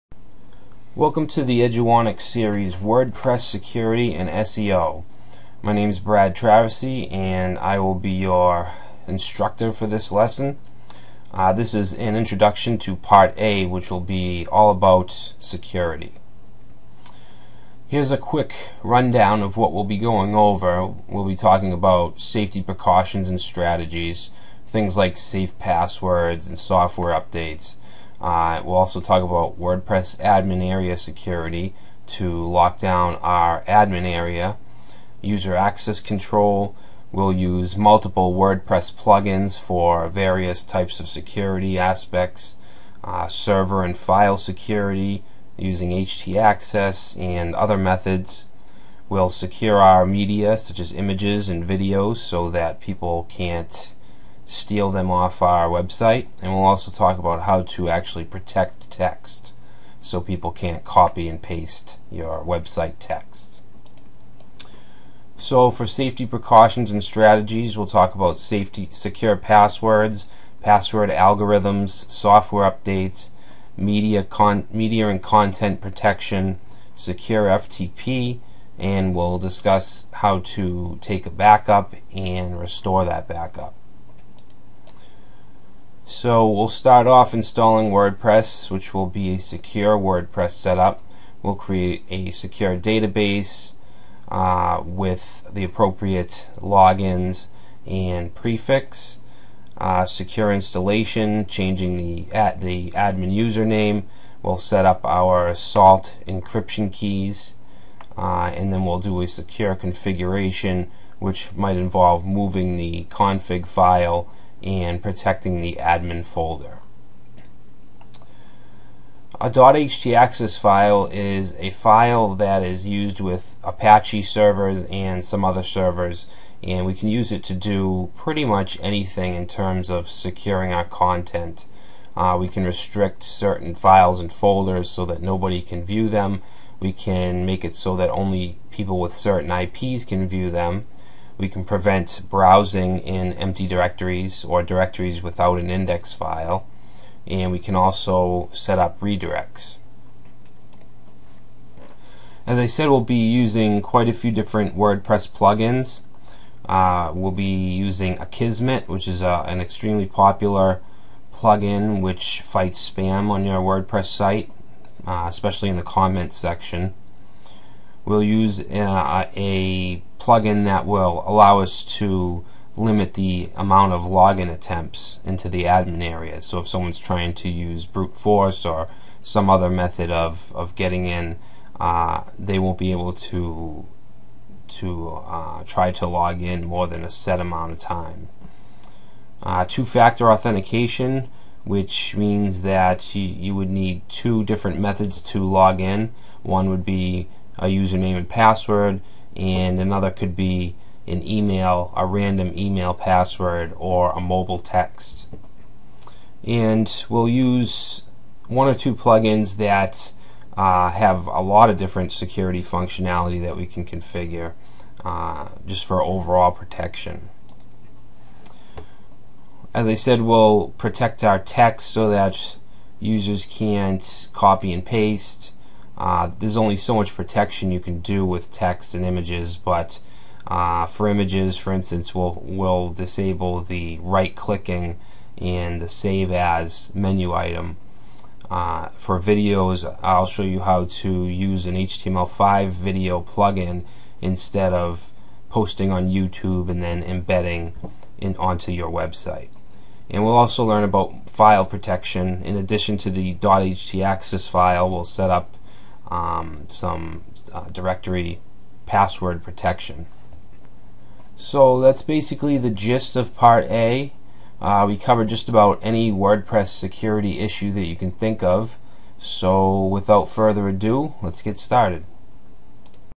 How to secure WordPress Website (part:1)
Course Name: Learn Complete Wordpress Security.
Instructor: Eduonix Learning Solutions
This course bring together all the essential ingredients to build a secure WordPress website. This course teaches you various tips and tricks which ensures that you website and your business is protected from malicious hackers. The course presents the contents in a easy to understand way and does not assume any technical knowledge.

This quick course will help you secure your website assets and will update with the latest tactics and strategies for WordPress Security.

Learn to Secure WordPress Websites
Learn strategies to prevent hacking attacks
Learn advance tactics for securing WordPress

Any WordPress user.